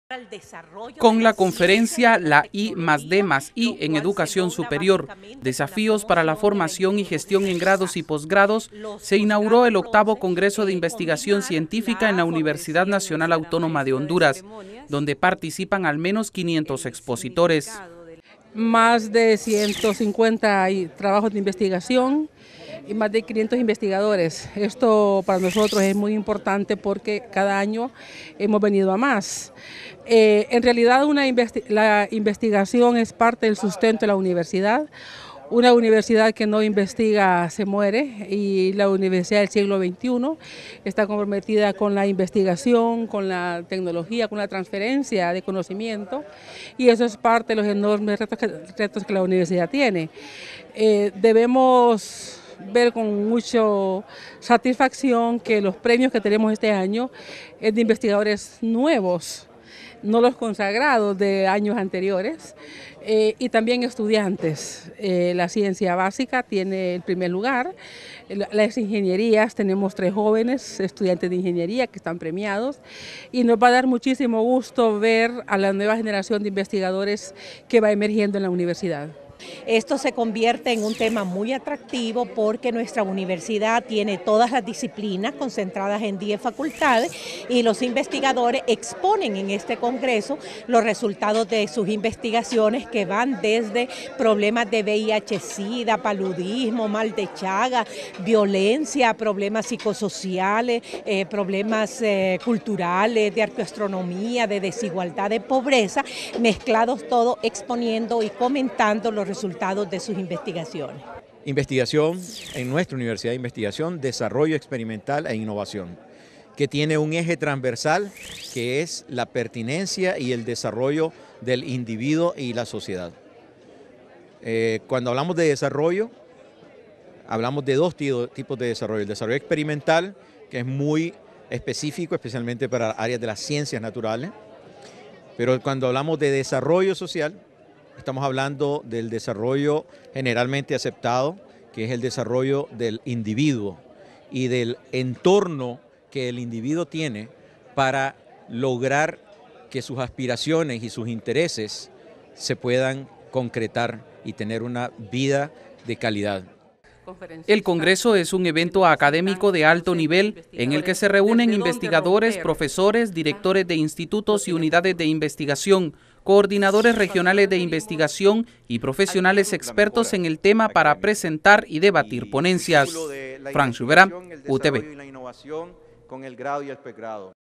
UTV: 8vo. Congreso de Investigación Científica en la UNAH
Más de 150 trabajos de investigación se expusieron y alrededor de 500 investigadores participaron en el "8vo. Congreso de Investigación Científica" desarrollado en la UNAH.